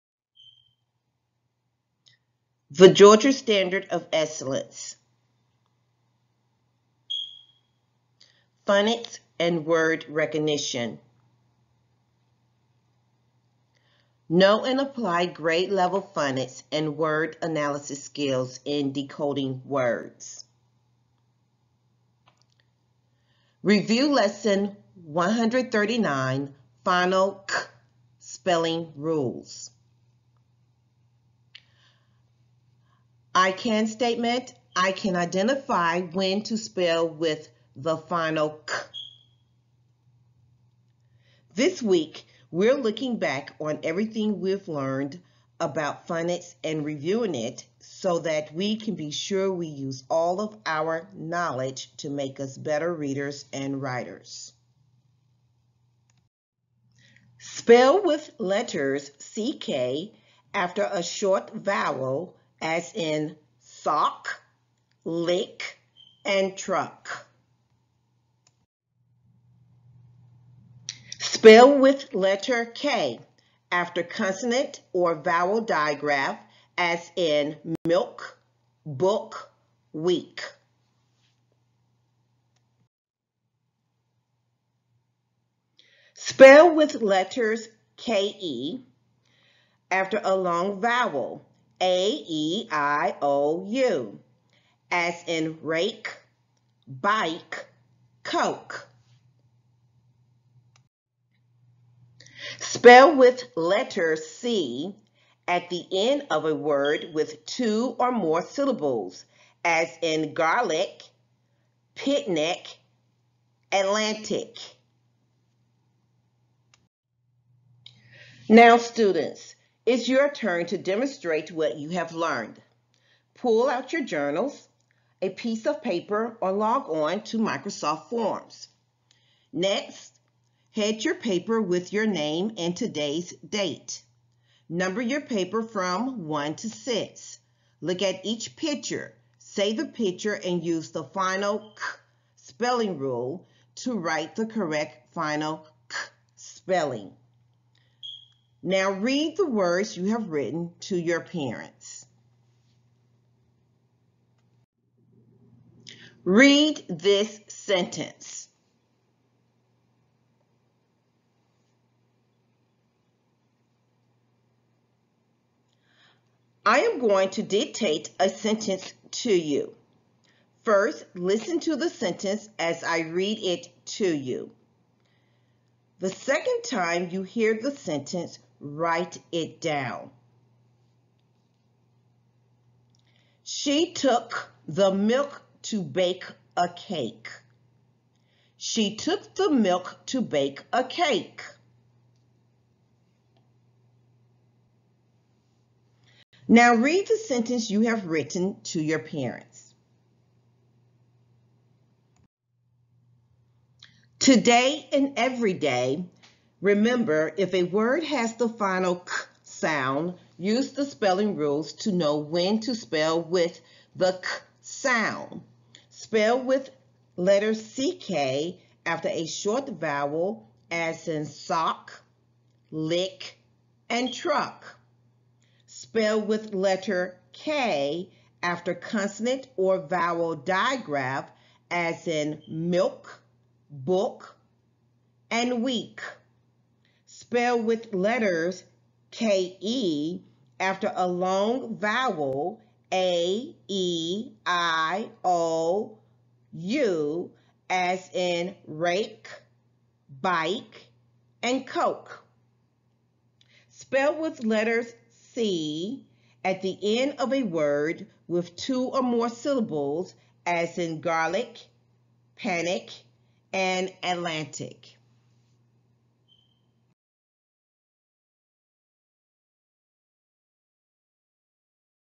1st Grade, Phonics, Week 3 Day 2: Final /k/ Spelling Rules
Phonics:  Final /k/ Spelling Rules
ck after a short vowel
k after consonant or vowel digraph
ke after a long vowel
c at the end of a word with two or more syllables